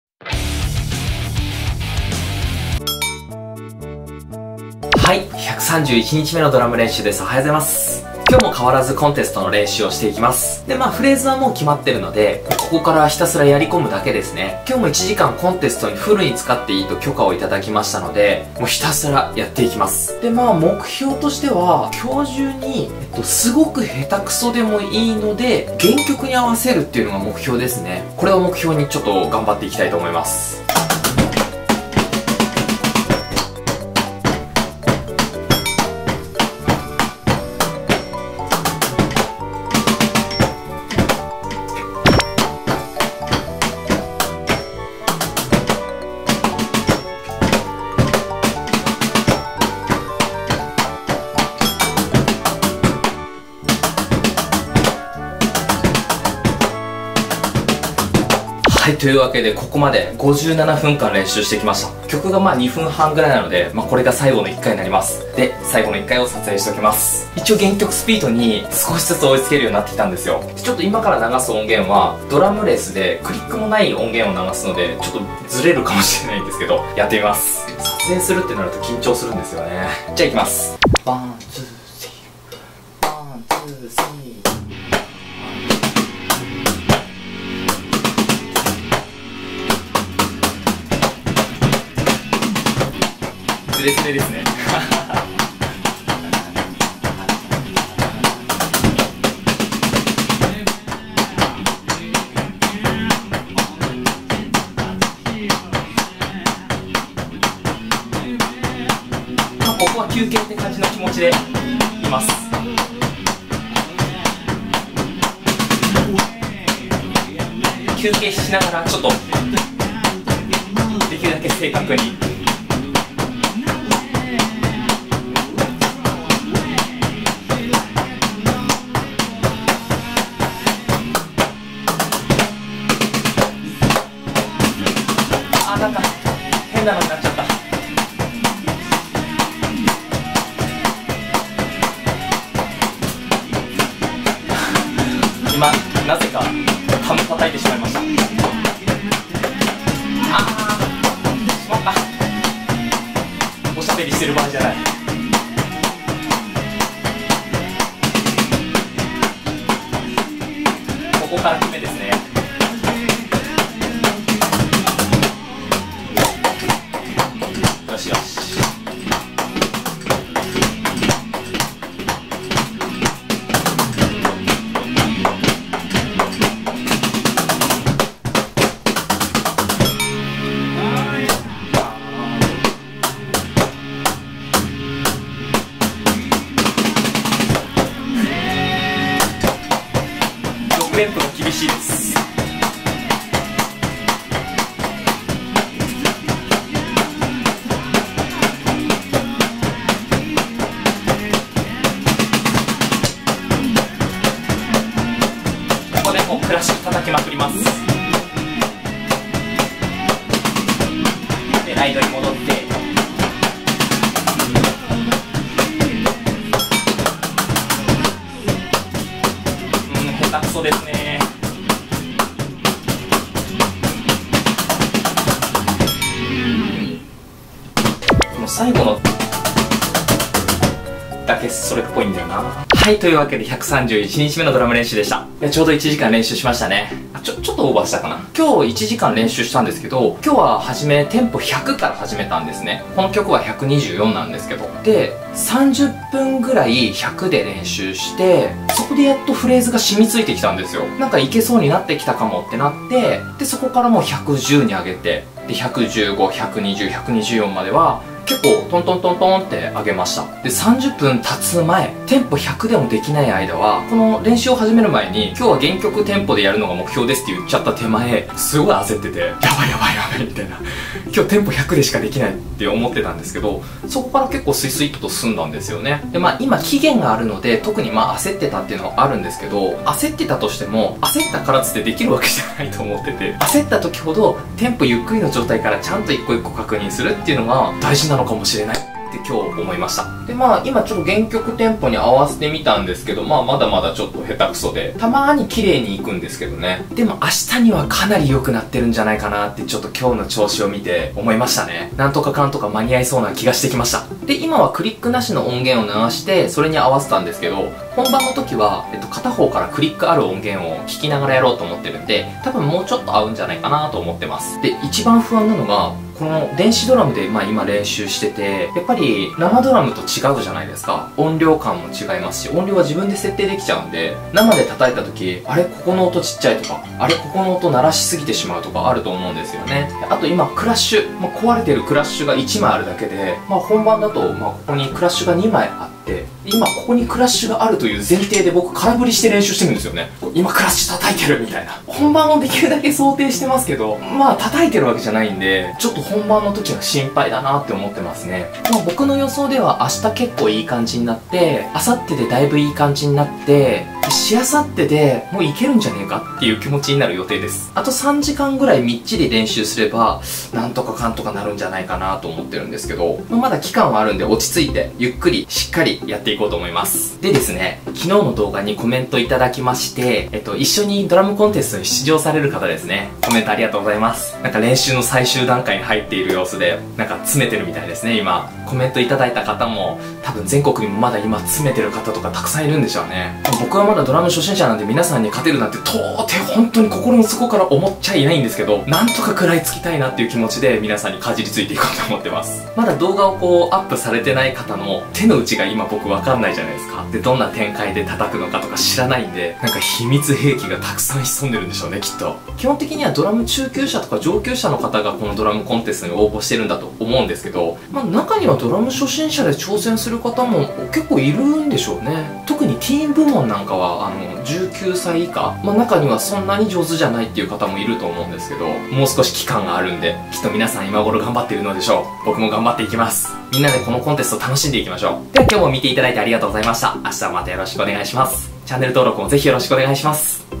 遂に原曲テンポ・スピードで叩けました！(ドラムコンテスト練習)
【ドラムチャレンジの趣旨】
毎日1時間ドラムを練習したら、1年後にどれくらい上手くなるのか試してみました。
その結果、「毎日練習すれば、1年後にはある程度ドラムを叩けるようになる」という結論が出ました。
これからドラムを始める方のモチベーションアップに役立てたら嬉しい限りです。
どうぞ、最終回も見てみて下さい！